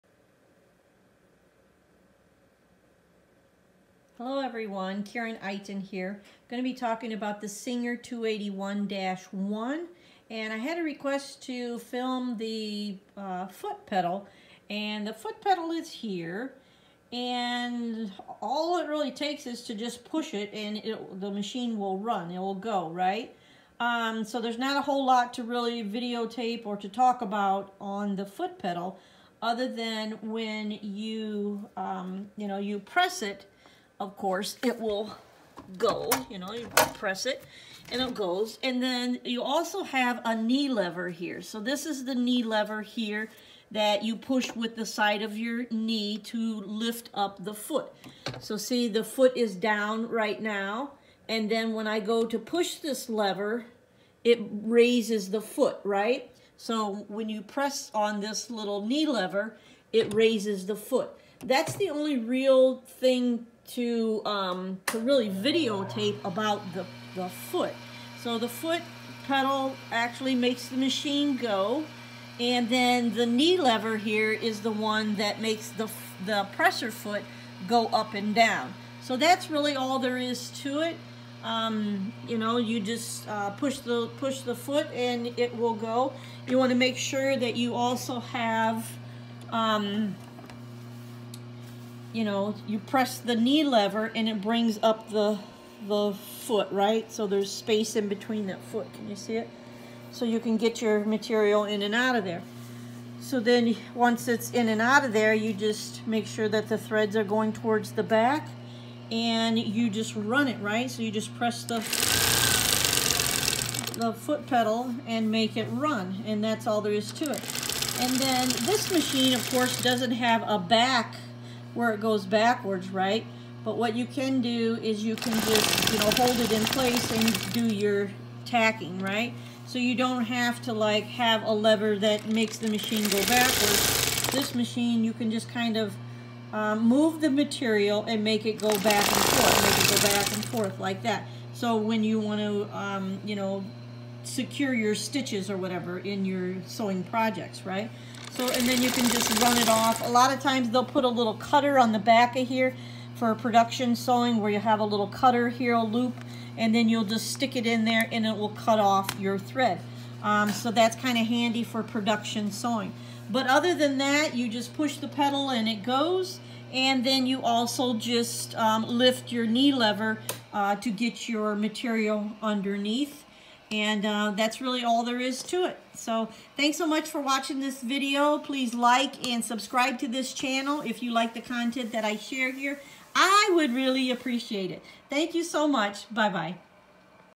Foot petal on a Singer 281-1 Sewing Machine
Showing you the foot petal and knee lever to run your machine.

Book your coaching call.

Tailoring book.